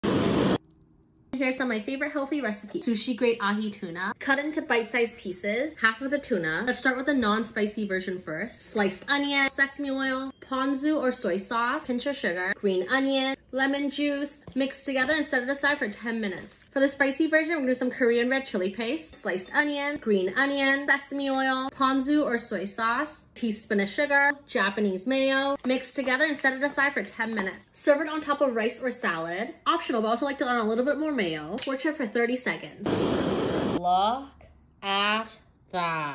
Which one do you prefer - spicy, creamy, or tangy, savory, and refreshing?
These Poke Bowl will be at every summer dinner party I host this year because they are delicious, easy to make, and a dish that EVERYONE will love—sharing two different versions of my homemade Poke Bowl today that take less than 20 minutes to make at home.

Serve is on top of a salad or a bowl of rice. Optional, but HIGHLY recommended; I love to torch the tuna to melts in my mouth and level up the umami flavor.

I had my first authentic Poke Bowl when I visited Hawaii a few years ago, and I instantly fell in love. Poke Bowls at Hawaii are honestly the best I’ve ever had. I have it every single day whenever I visit Hawaii. The seafood is so fresh, and the marinate is seasoned to perfection. Since we can’t travel right now, here is a recipe that I’ve been making for the past year that all my friends and family love.